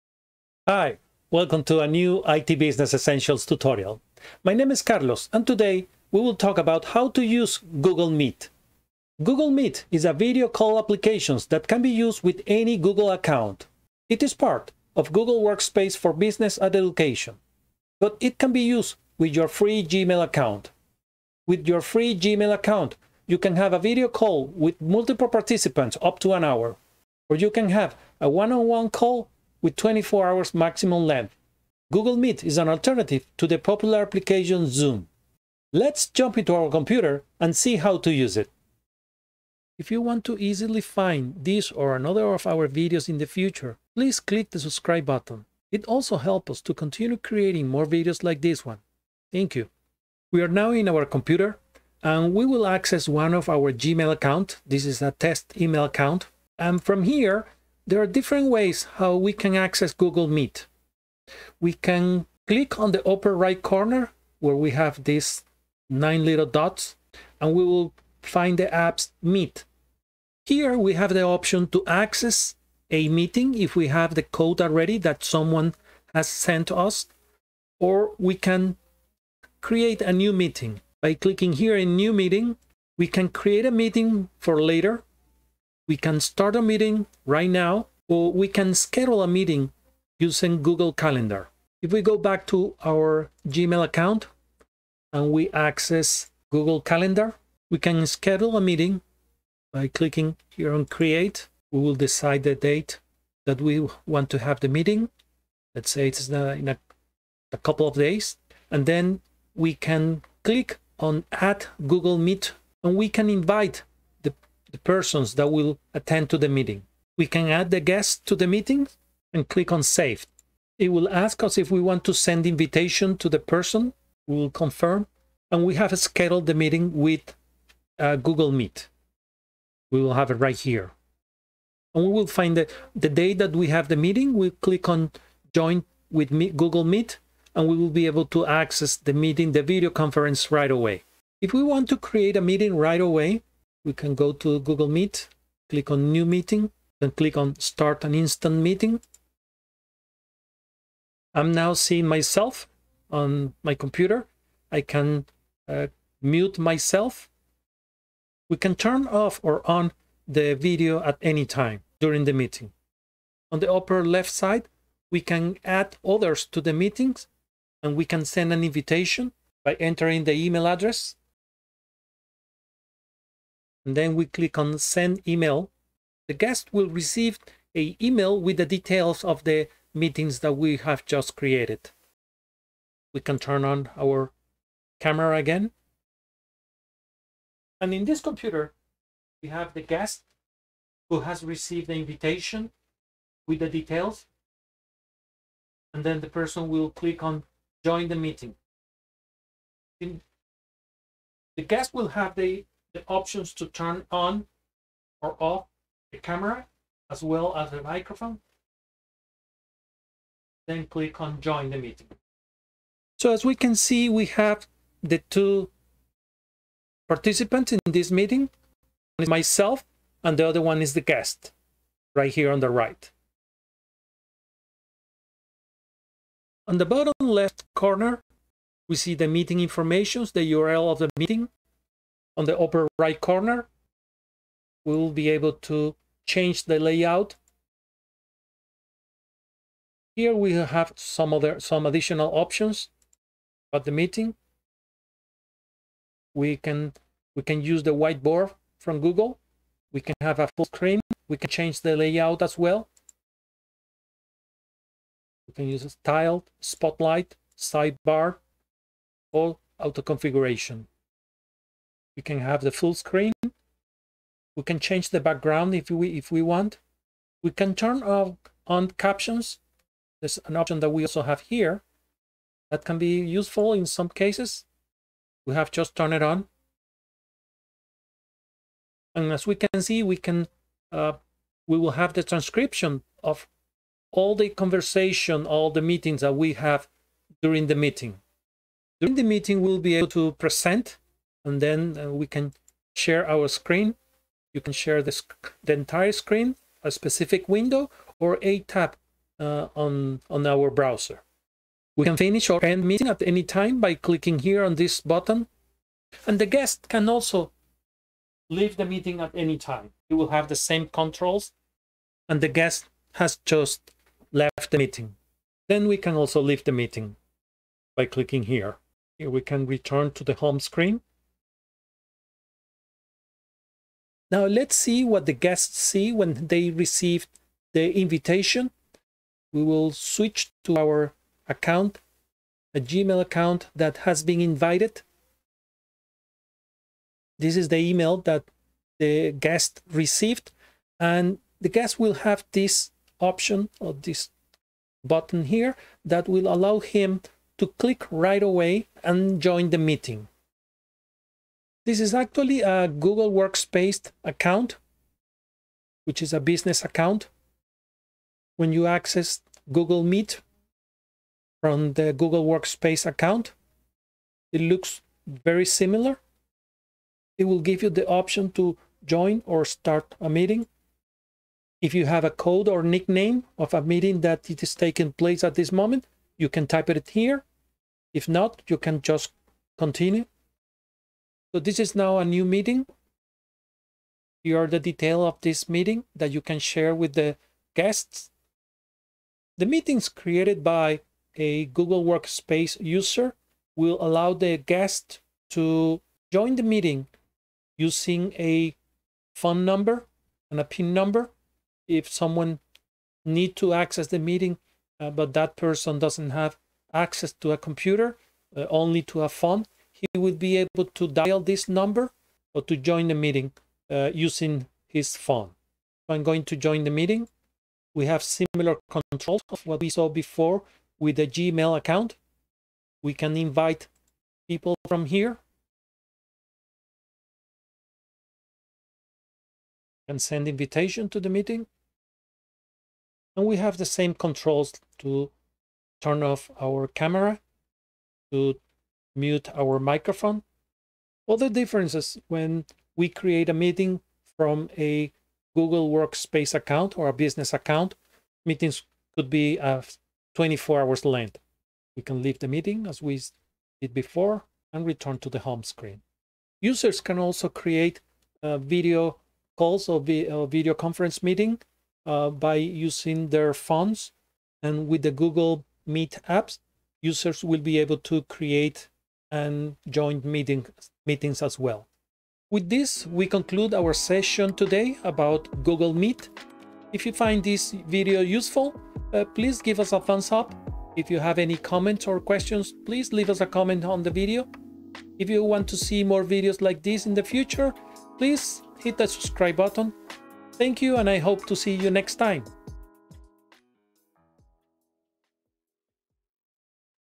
How to use Google Meet - Tutorial
This video describe how to access and use Google Meet app from a Free Gmail account as well as a Google Workspace account.

Here are the described processes:
- Access Google Meet
- How to schedule a Meeting from Google Calendar
- Create a instant meeting
- How to join a meeting
- How to change settings during a meeting
- How to turn transcript on and off
- How to leave a meeting
- How to access a meeting by dialing a phone number